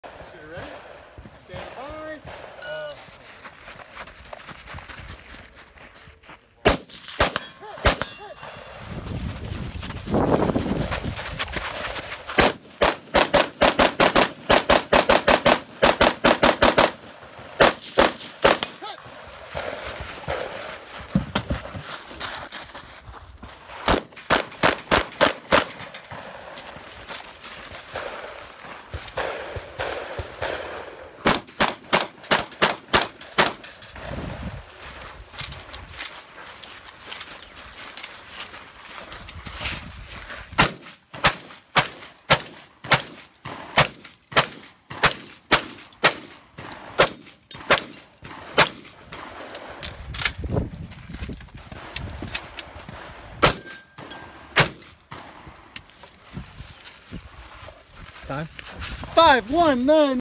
2019 Ohio State Multi Gun Championship Stage 2
Stage 2 from the 2019 Ohio State Multi Gun Championship.  9th place finish in Tac Ops. and High Law Enforcement.